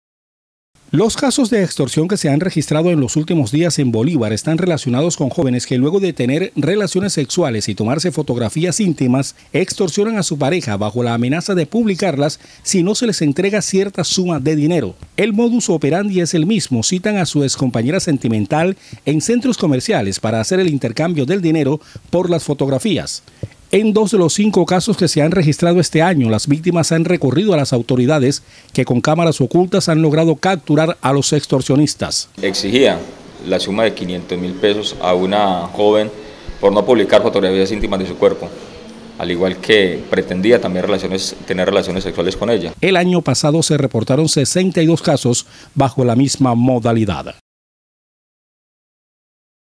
EXTORSIONABAN A SUS NOVIAS PARA NO PUBLICAR FOTOS
Los casos de extorsión que se han registrado en los últimos días en Bolívar están relacionados con jóvenes que luego de tener relaciones sexuales y tomarse fotografías intimas extorsionan a su pareja bajo la amenaza de publicarlas si no les entregan cierta suma de dinero.

El modus operandi es el mismo, citan a su excompañera sentimental en centros comerciales para hacer el intercambio del dinero por las fotografías.

En dos de los cinco casos que se han registrado este año las víctimas han recurrido a las autoridades que con cámaras ocultas han logrado capturar a los extorsionistas. El año pasado se reportaron 62 casos bajo la misma modalidad.